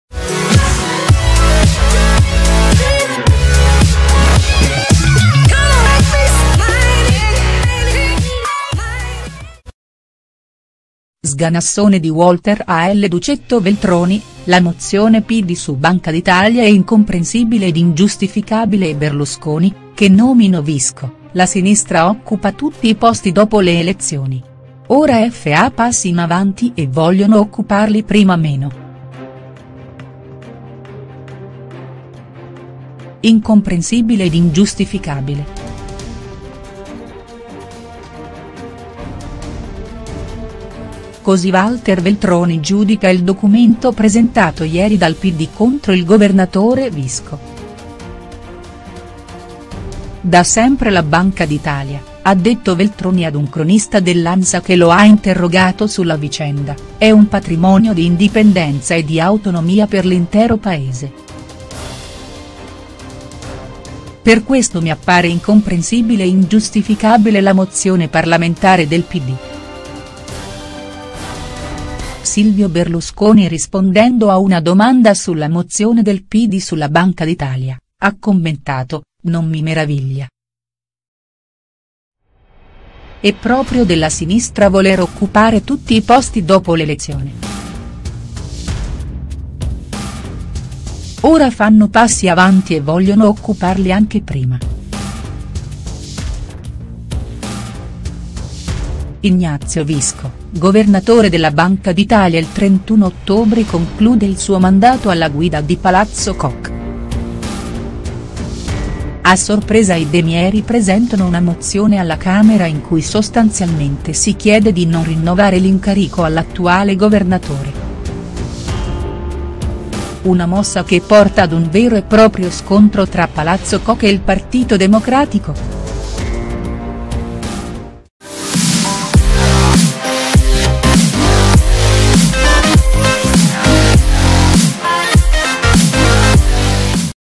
SGANASSONE DI “UOLTER” AL DUCETTO – VELTRONI: LA MOZIONE PD SU B.... -I STARS NEWS
Da Ansa   gentiloni veltroni renzi "Incomprensibile ed ingiustificabile". Così Walter Veltroni giudica il documento presentato ieri dal Pd contro il governatore Visco. "Da sempre la Banca d'Italia - ha detto Veltroni ad un cronista dell'Ansa